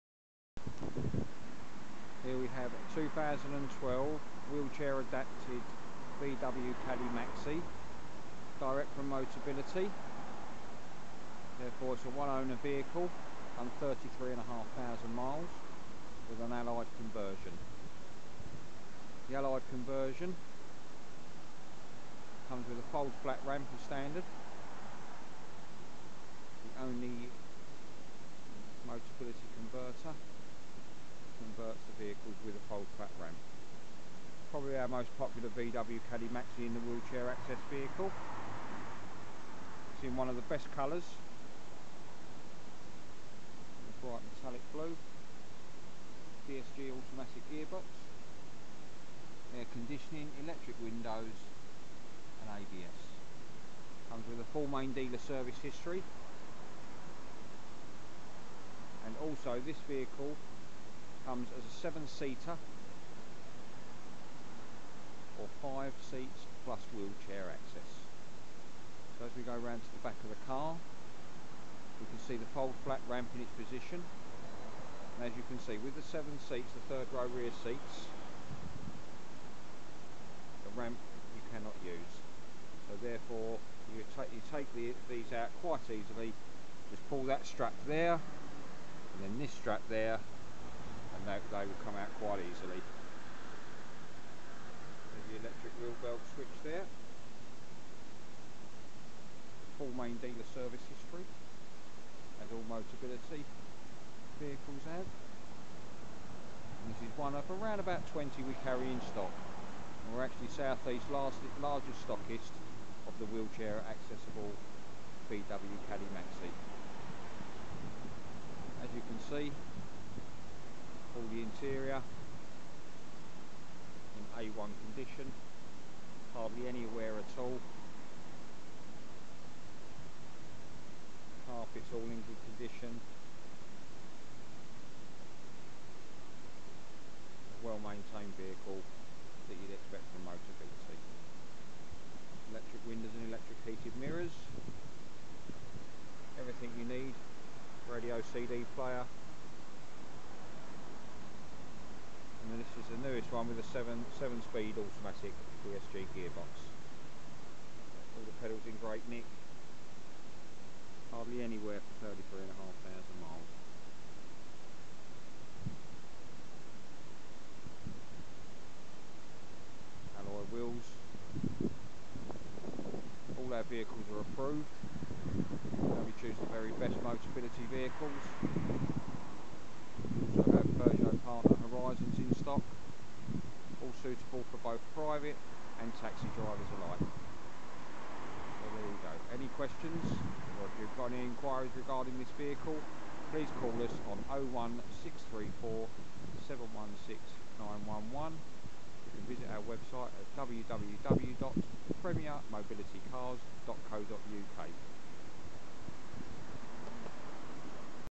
7 x seat Wheelchair adapted VW Caddy Maxi Life T: 01634 716911
We are South East England's largest stockist of wheelchair adapted VW Caddy Maxi's. This particular Caddy is a 7 seat or 5 plus wheelchair vehicle with an Allied conversion. One of the main features of this car is it's fold flat ramp. The VW Caddy For more info on this, or any of our other wheelchair adapted VW Caddy Maxi's, please either visit us or if you prefer to call us on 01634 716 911